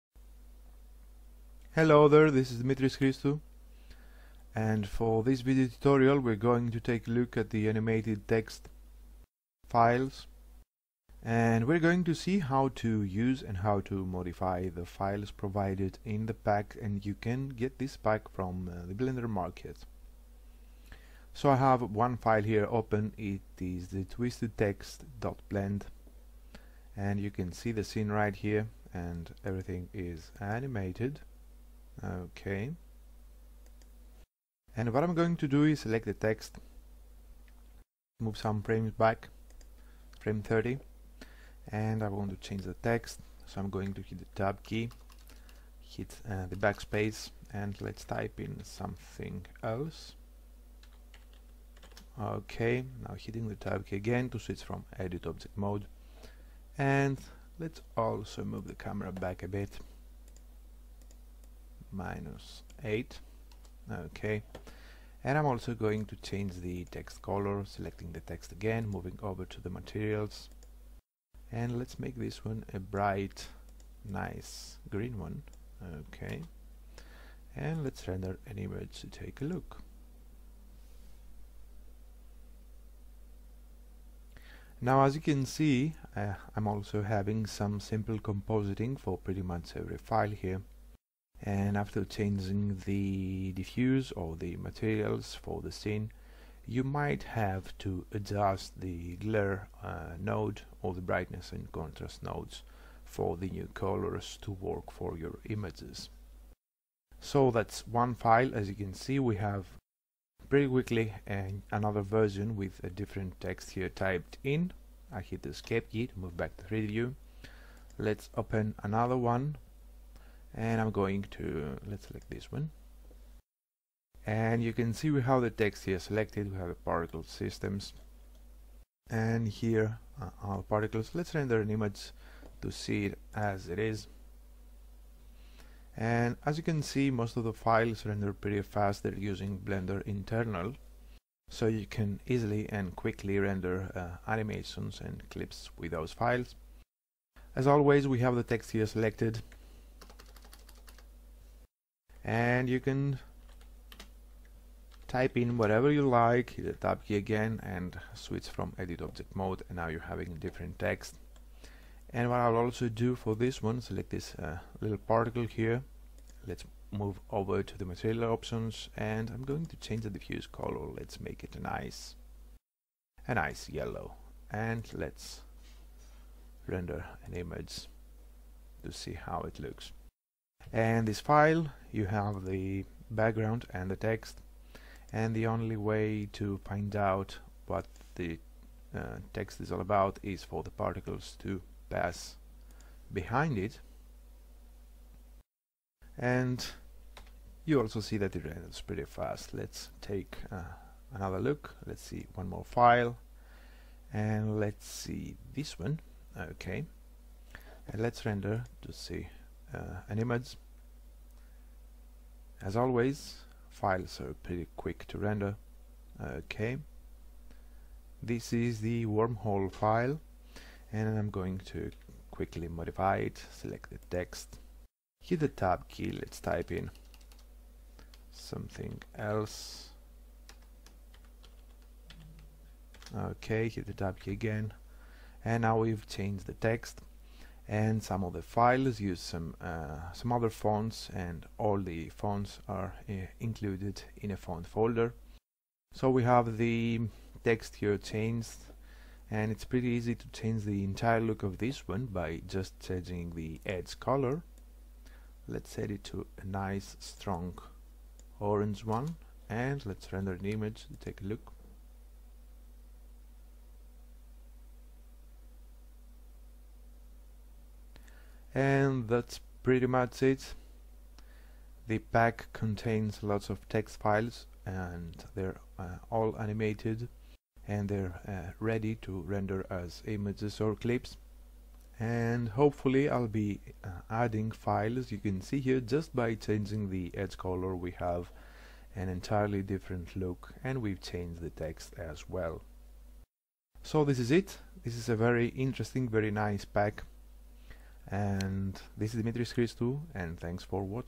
BM: Animated Text Tutorial
A small tutorial showing and explaining how to use and modify the Animated Text pack files sold over at Blender Market. Hope it answers some of your questions.